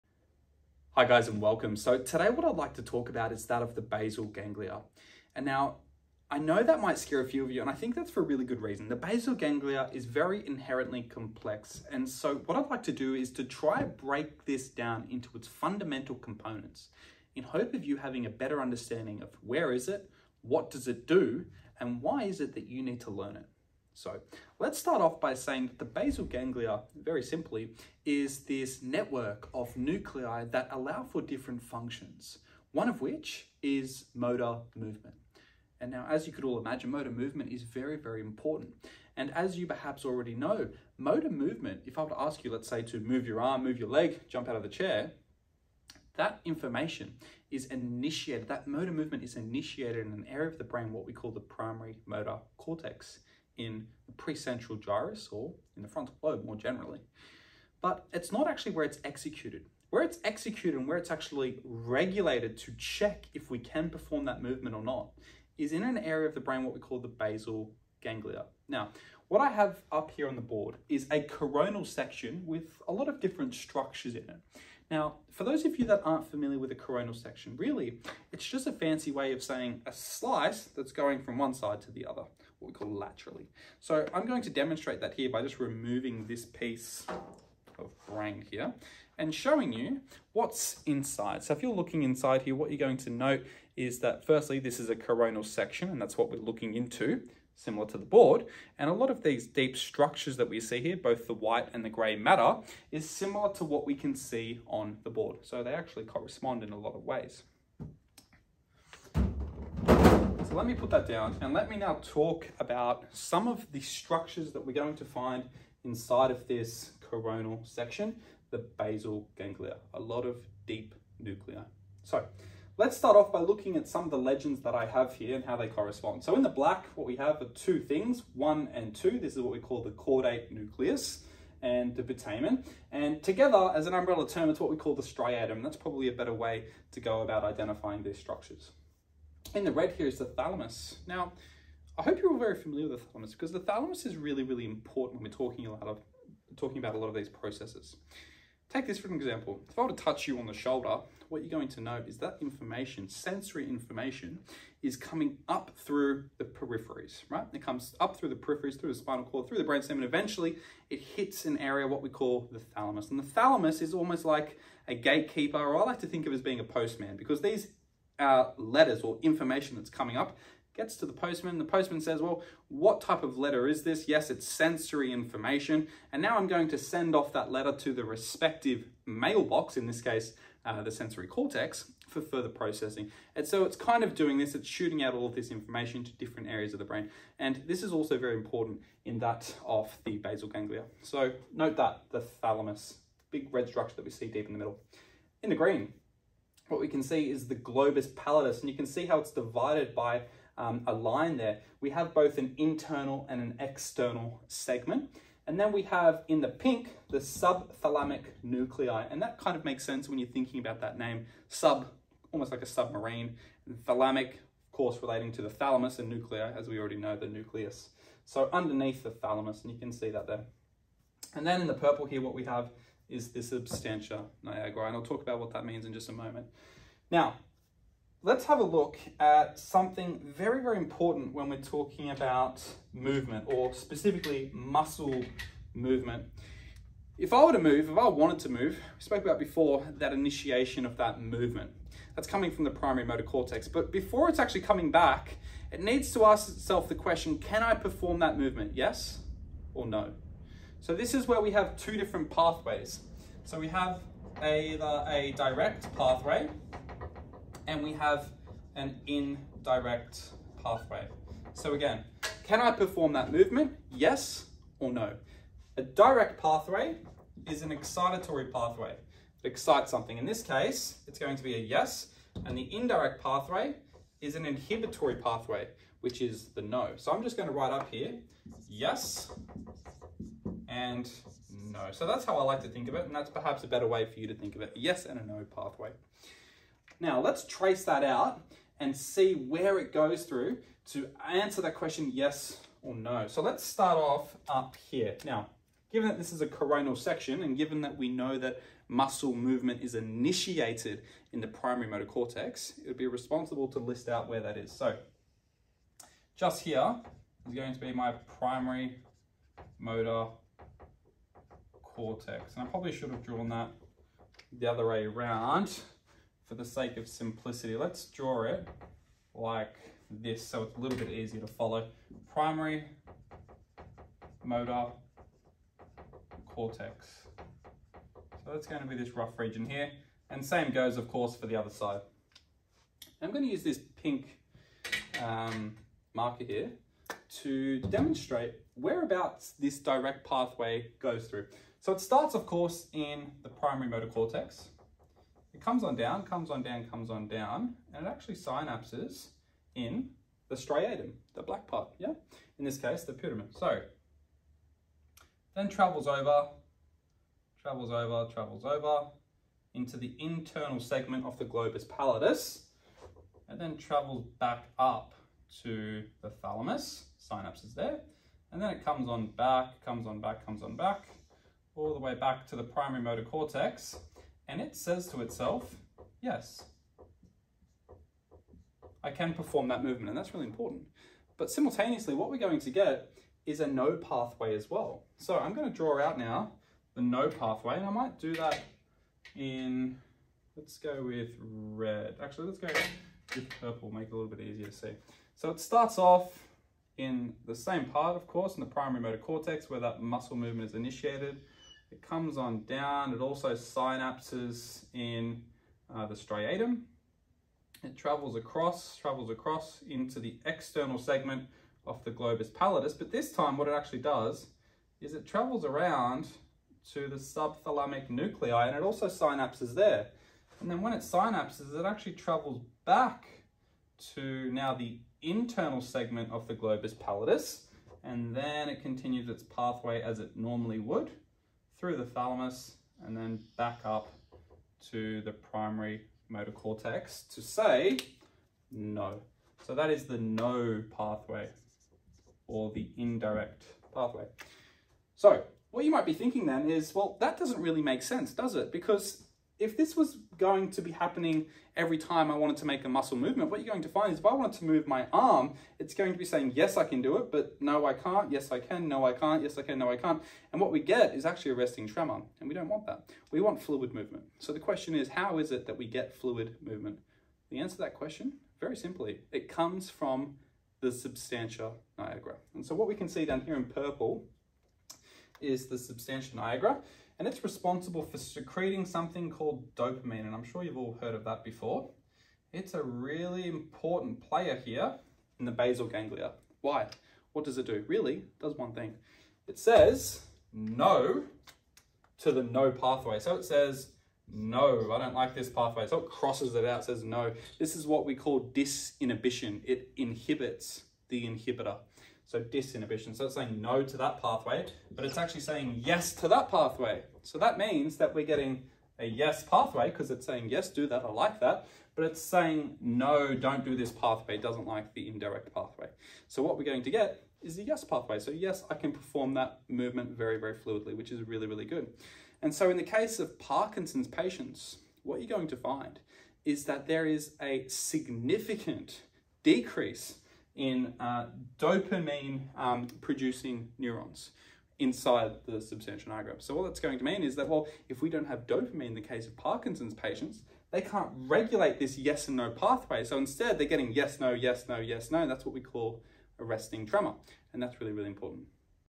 The Basal Ganglia
A quick review on the structure, function and pathways of the basal ganglia and how this regulates motor movement. This video also touches on the importance of dopamine and it’s correlation to Parkinson’s disease.